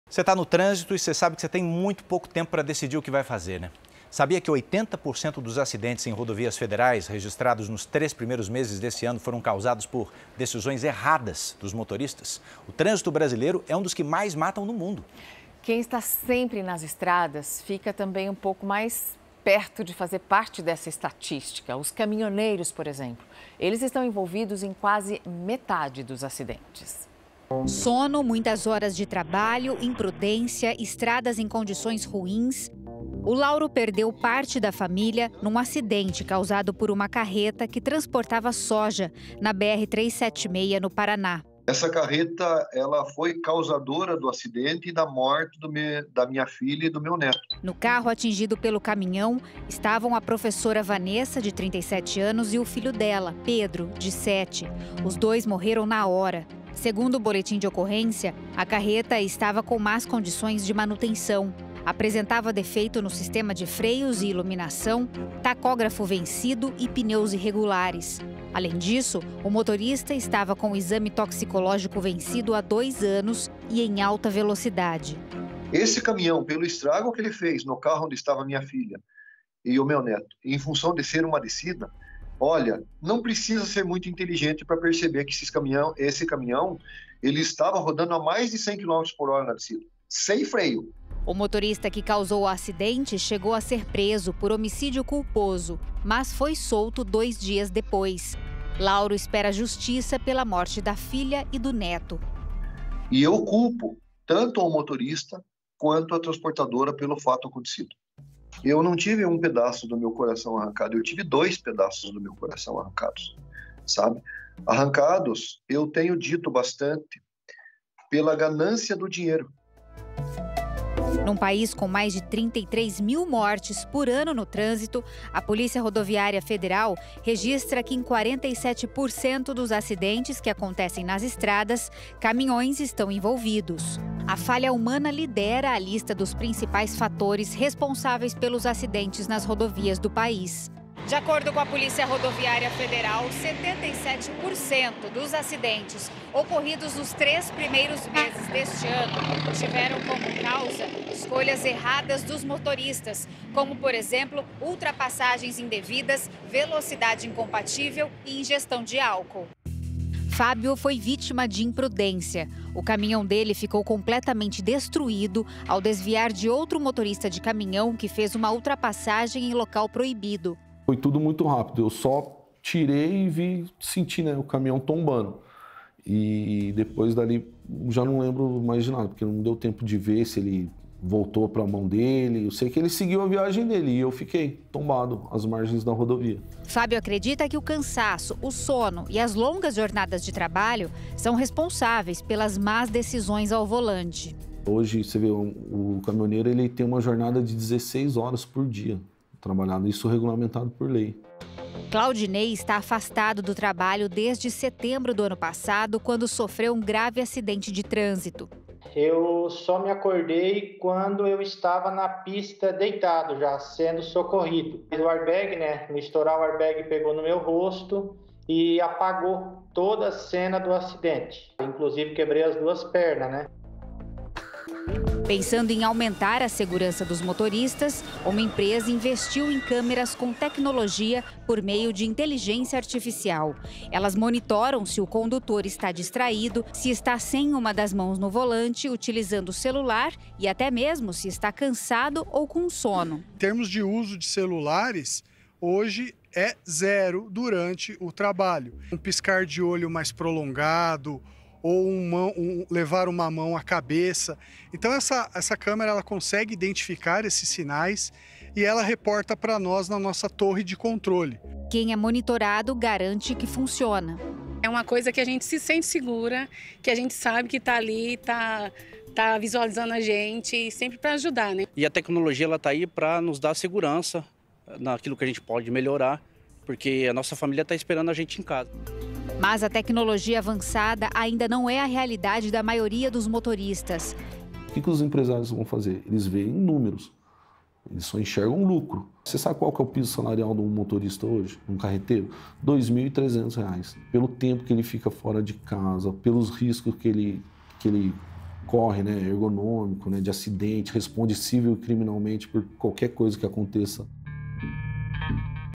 Caminhoneiros estão envolvidos em metade dos acidentes de trânsito nos primeiros meses de 2023
Quase 80% dos acidentes em rodovias federais registrados nos três primeiros meses desse ano foram causados por decisões erradas dos motoristas. O trânsito brasileiro é um dos que mais matam no mundo. Quem está sempre nas estradas fica mais perto de fazer parte da estatística. Os caminhoneiros estão envolvidos em quase metade dos acidentes.